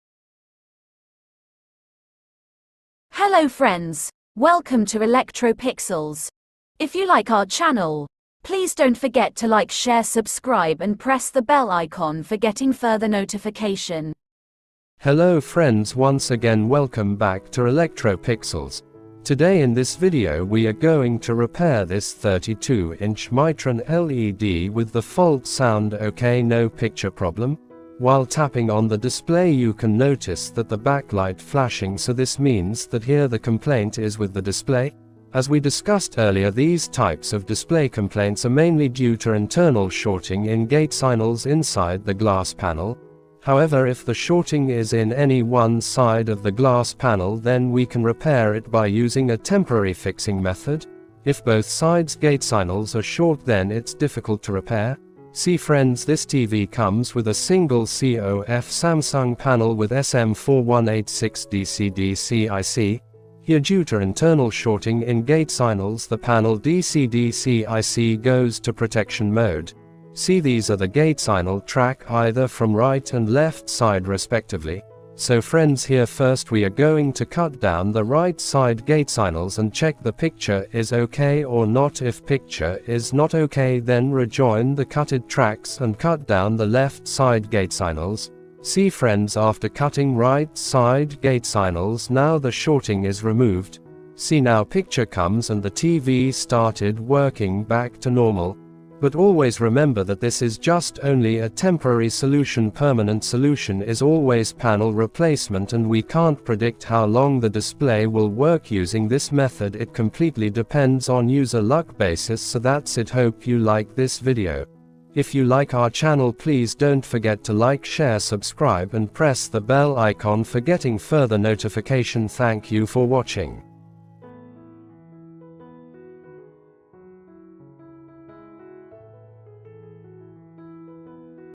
Panel Short Removing in Mytron Led Tv with SM4186 Dc to Dc Ic |Fault Blank display |No vgh,vgl, avdd
Panel Short Removal in Mytron Led Tv with SM4186 Dc to Dc Ic |Fault Blank display |No vgh,vgl, avdd